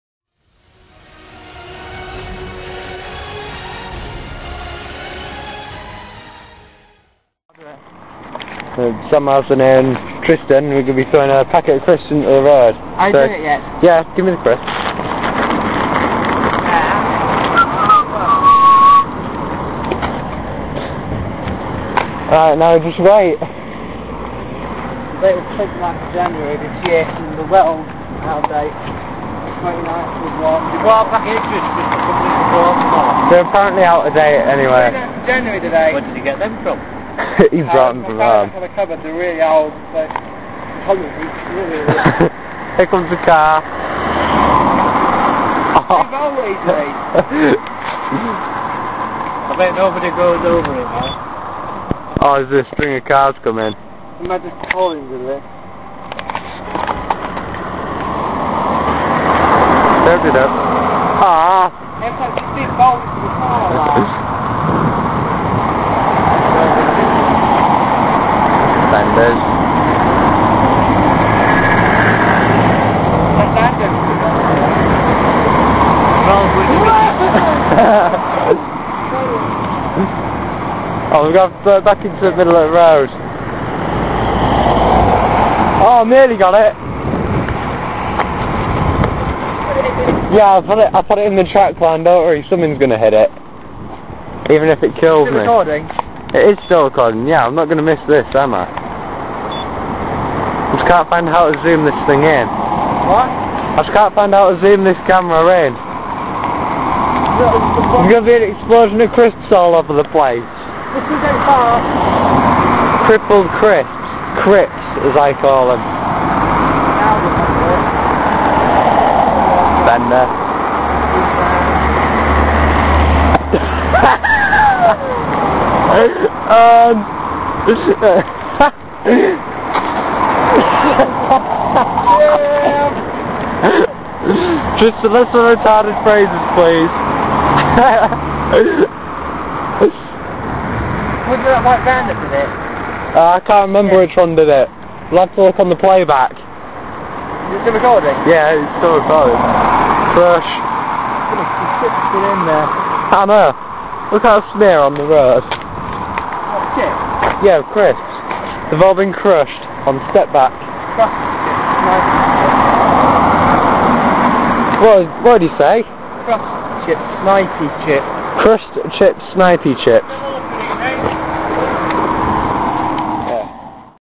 Crushing Crisps with Cars
My friend tristan thought it would be funny to throw an out of date packet of crisps into the main road of our town while we wait for the bus to our youth group. I had a camera on me and when he saw it was adamant this should be recorded. Random sense of homour? This is your kind of video :)